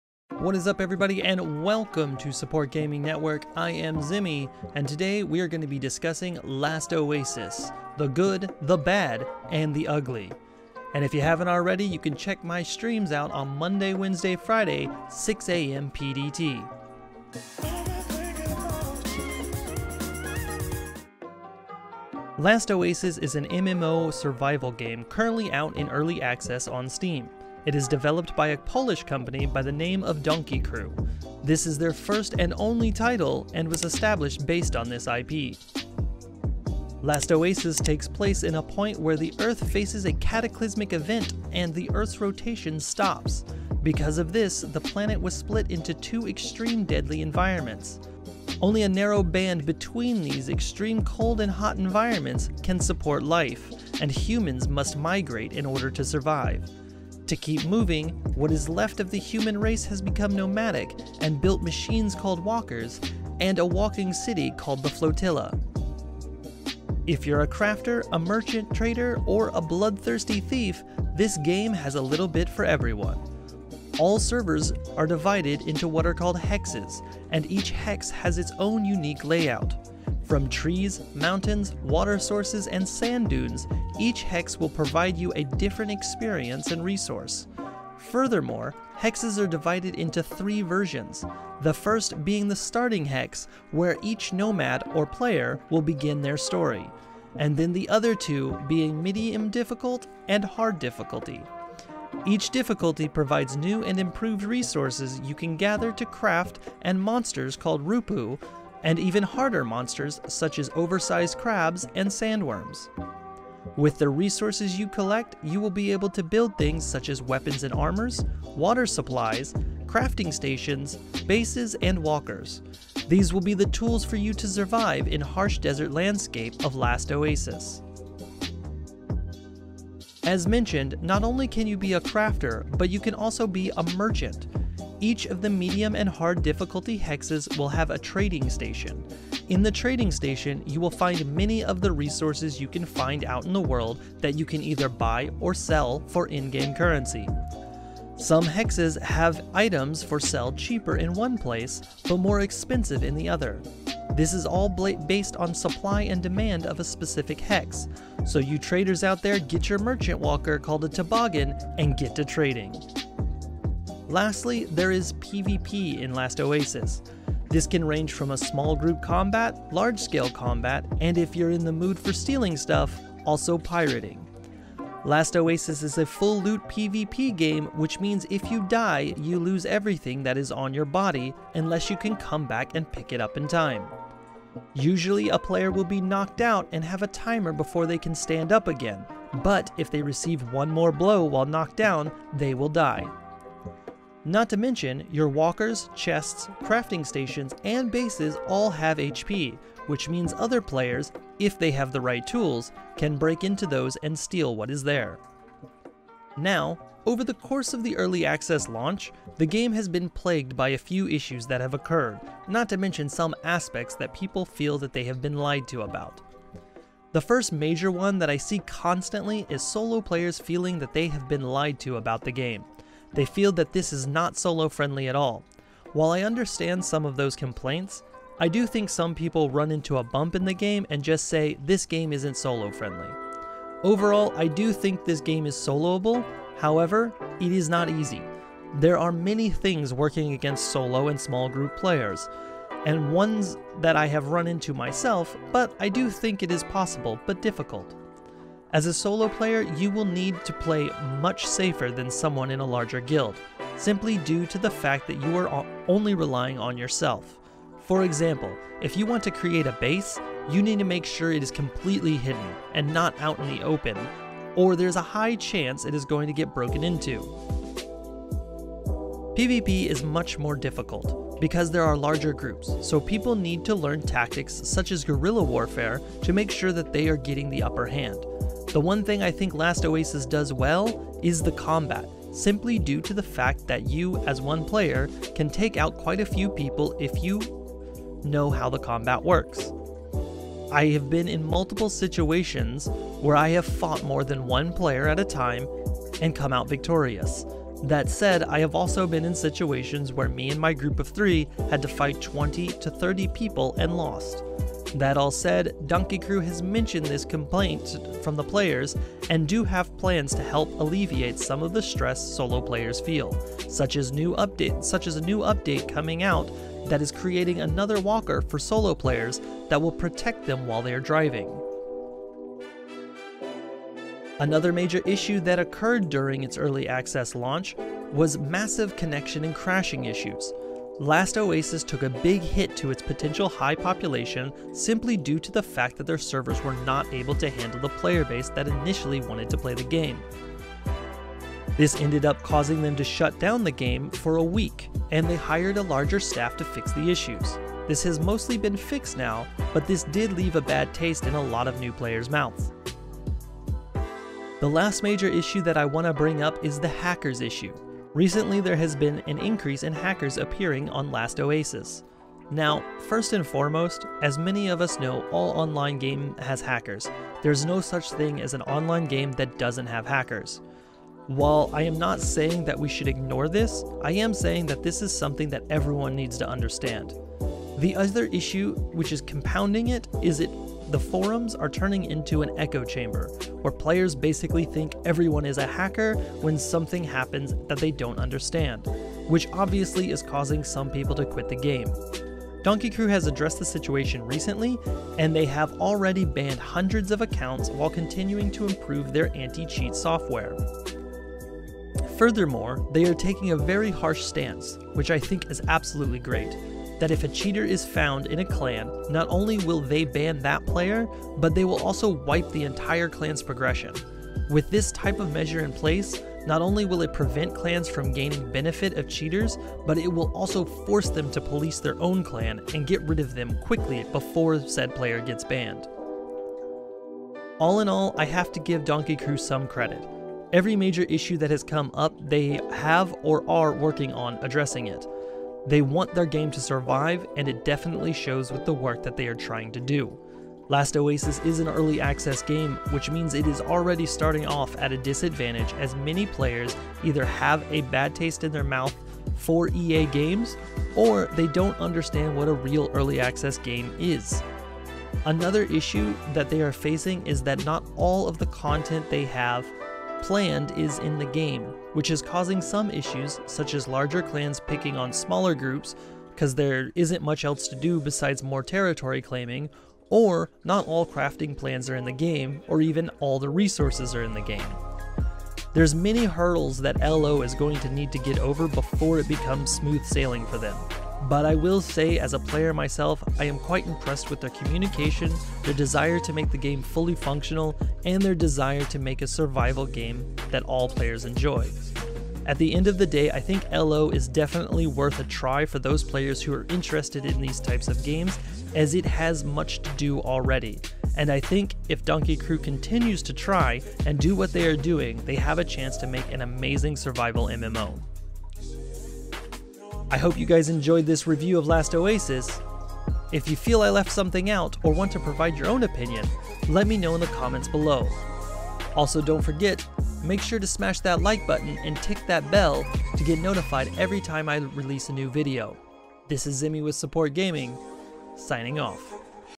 Last Oasis - Is it Worth It? (Game Review 2020)
What is up everyone and welcome to the Support Gaming Network, I am Zemi and today we will be discussing Last Oasis! The good, the bad, and the ugly.

Last Oasis is an MMO Survival Game currently out in Early Access on Steam. It is developed by a Polish company by the name of Donkey Crew. This is their first and only title and was established based on this IP.

Last Oasis takes place in a point where the Earth faces a cataclysmic event and the Earth's rotation stops. Because of this, the planet was split into two extreme deadly environments. Only a narrow band between the extreme cold and hot environments can support life, and humans must migrate in order to survive. To keep moving, what is left of the human race have become nomadic and built machines called walkers and a city called the Flotilla.